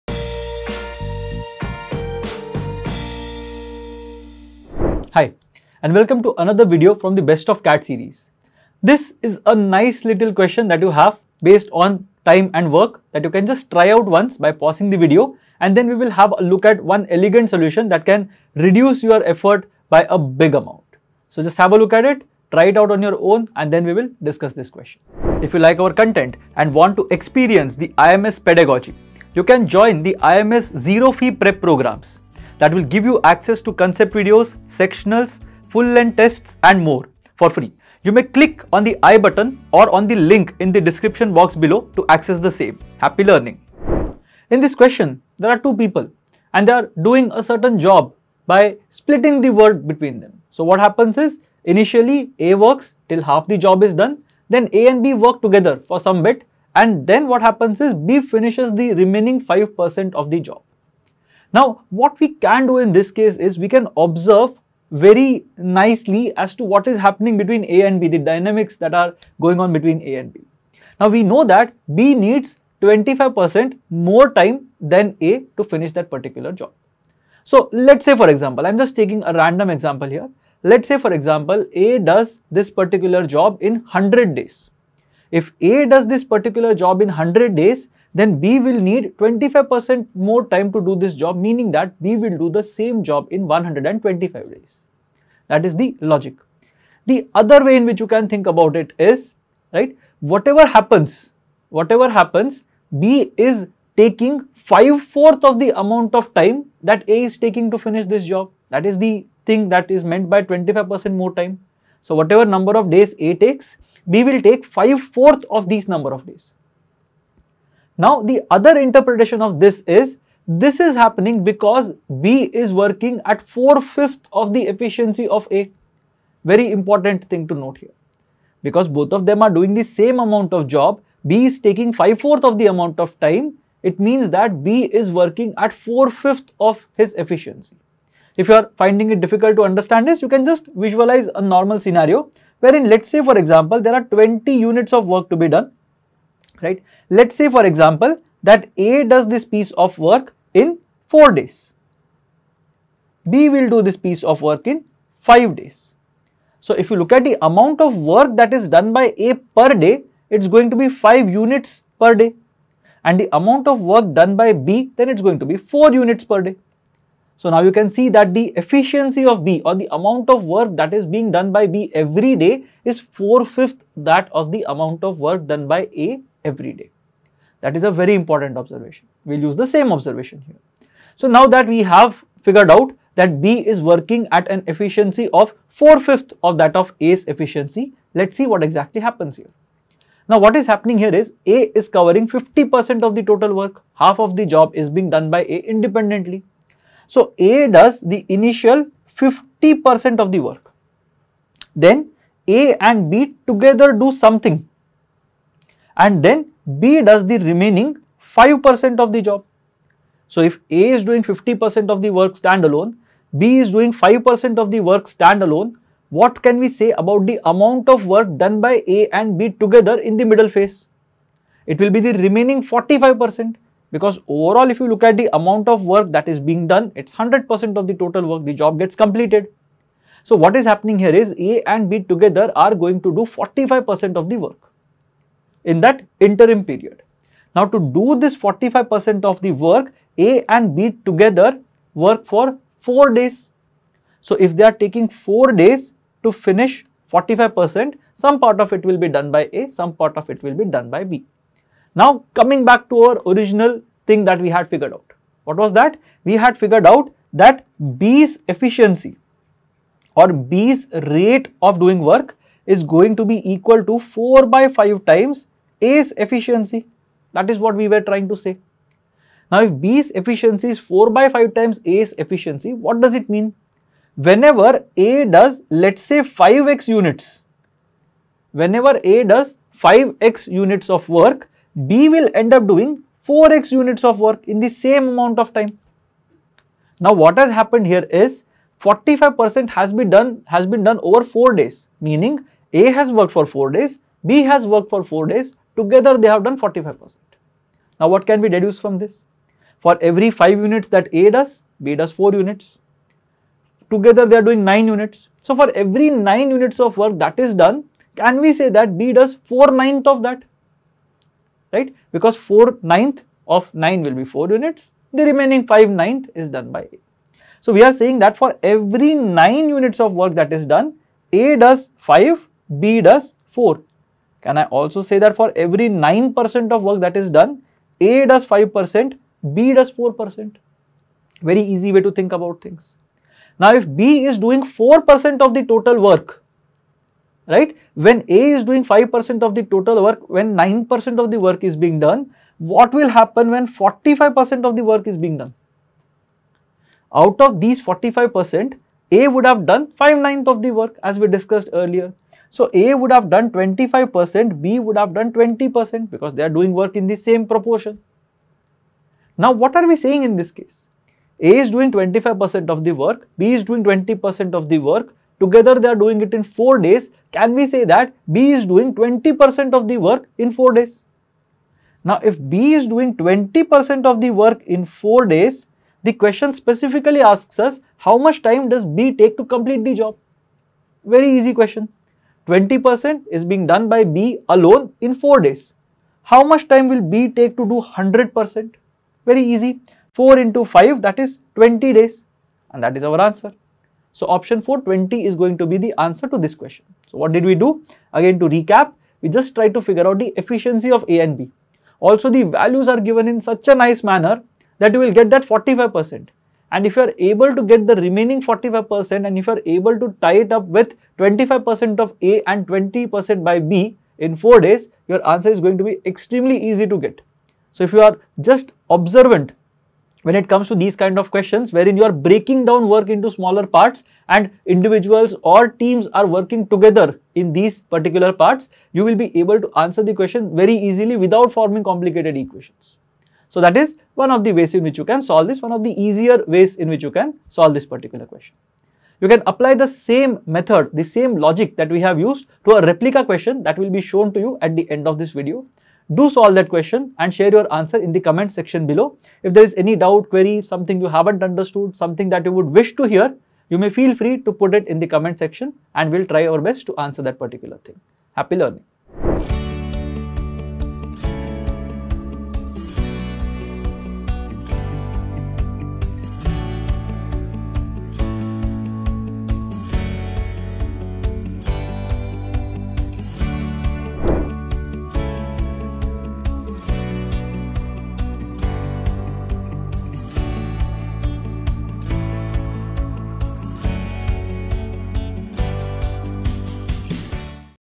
Arithmetic for CAT 2025 Preparation | Best of CAT | CAT PYQs with Solution | Shashank Prabhu
Gear up for CAT 2025 with essential arithmetic tips and tricks! Master key concepts and solve challenging quant questions with ease.